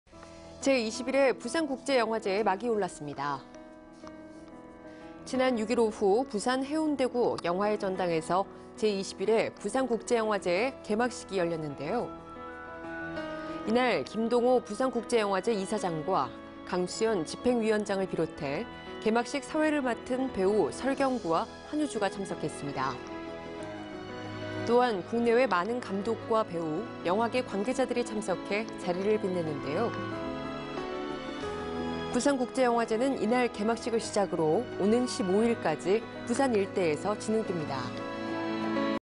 [생방송 스타뉴스] 제21회 부산국제영화제 개막, 15일까지 열려
제21회 부산국제영화제의 막이 올랐습니다.

지난 6일 오후 부산 해운대구 영화의전당에서 제21회 부산국제영화제의 개막식이 열렸는데요.

이날 김동호 부산국제영화제 이사장과 강수연 집행위원장을 비롯해 개막식 사회를 맡은 배우 설경구와 한효주가 참석했습니다.

또한 국내외 많은 감독과 배우, 영화계 관계자들이 참석해 자리를 빛냈는데요.

부산국제영화제는 이날 개막식을 시작으로 오는 15일까지 부산 일대에서 진행됩니다.